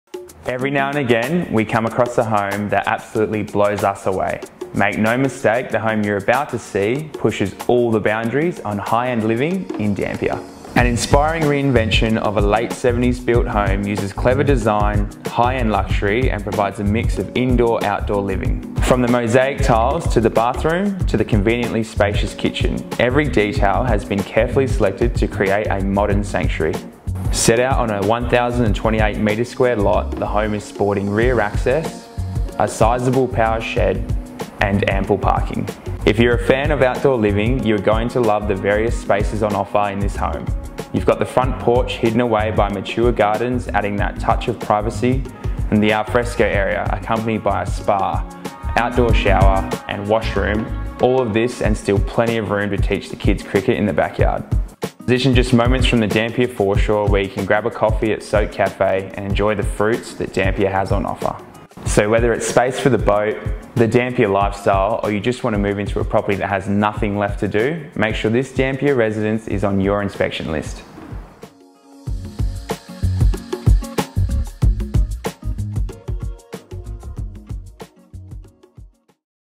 Realmark Karratha Presents 6 Tachikawa Court, Dampier
Situated in the coastal town of Dampier, this 3-bedroom home encapsulates high-end living. Featuring wood floors throughout, a mosaic tiled bathroom, and a spacious, open plan kitchen, this property must be seen to be truly appreciated.

Call James Group on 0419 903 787 to view the property and find out more